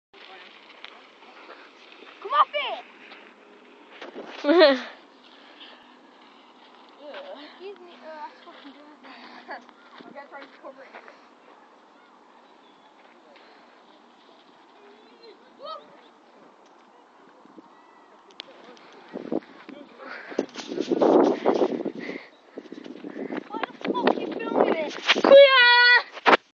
dirt track fail at higgs
connel tried showing off and this is what happens when you show off.